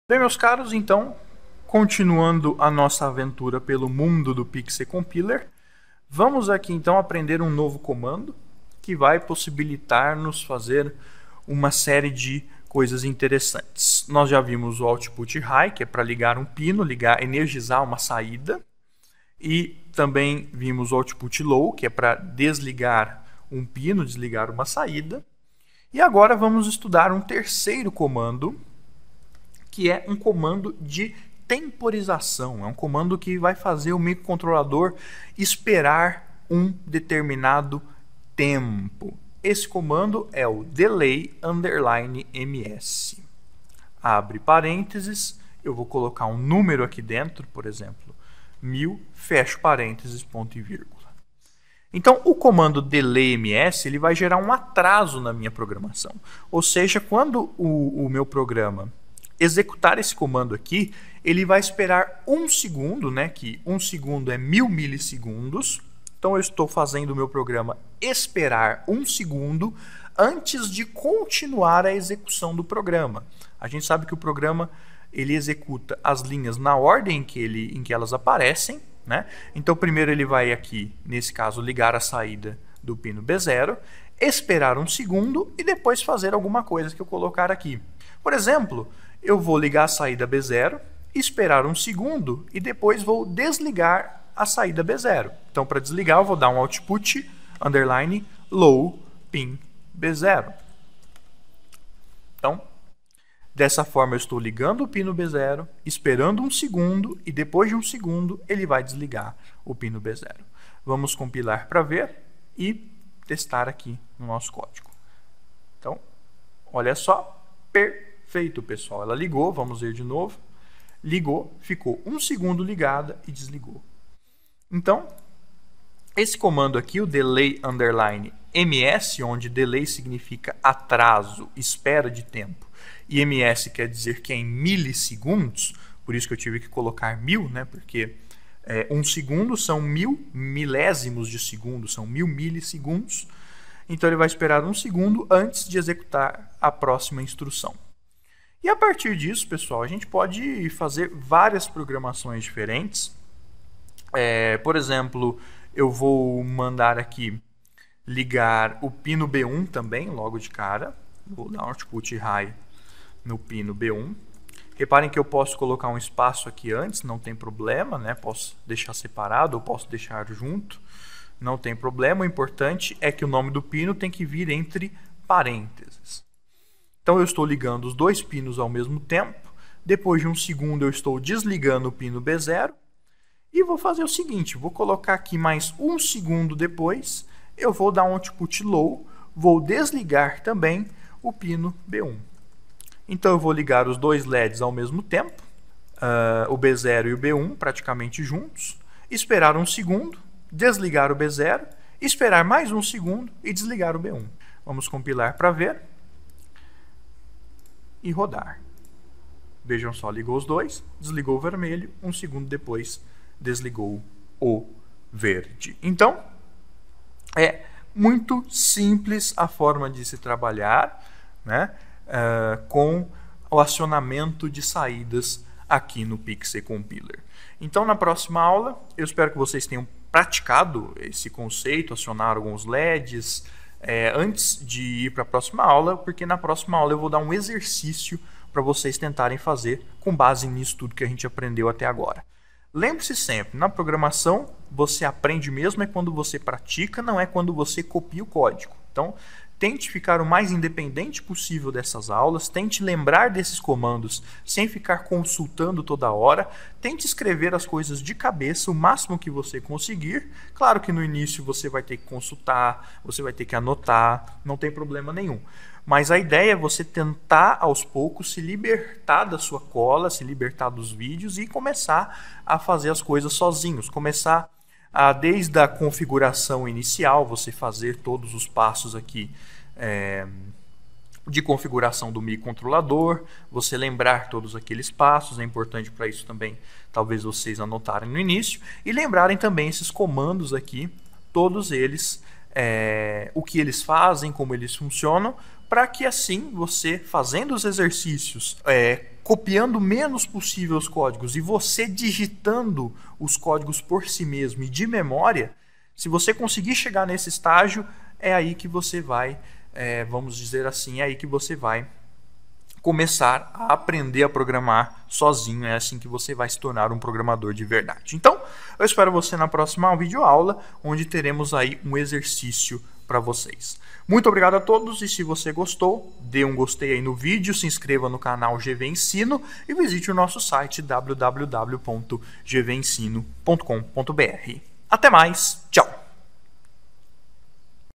PIC #7: [CCS] Temporização e dicas para quem está começando
Neste vídeo, vamos aprender como realizar a temporização no CCS PIC C Compiler. Para isso, utilizaremos leds e o Microcontrolador PIC16F88. Nesta aula, você também terá algumas dicas de programação para novatos. Bons estudos!